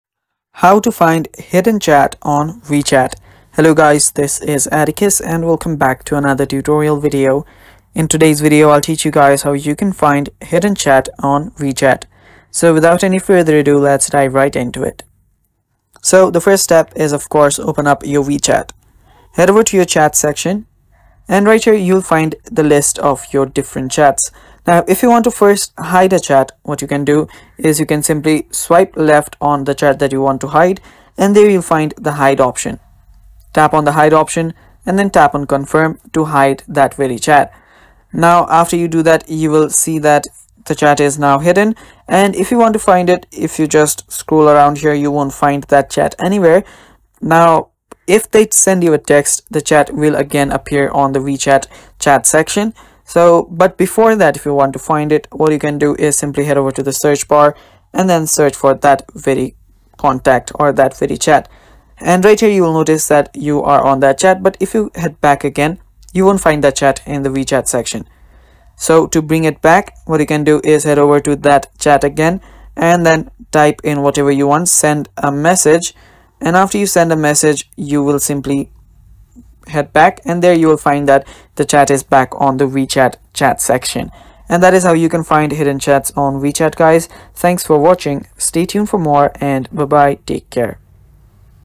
How to Find Hidden Chat on WeChat
Looking for hidden chats on WeChat? This video will guide you through the steps to find hidden conversations on WeChat quickly and easily. Whether you're asking, "How do I access hidden chats on WeChat?" or need help recovering archived messages, this tutorial has you covered. Learn how to locate, unhide, and manage hidden chats. Watch now to see how you can find hidden chats on WeChat today!

Timestamps:
00:00 How to Find Hidden Chat on WeChat
00:10 How Do I Unhide Chats on WeChat?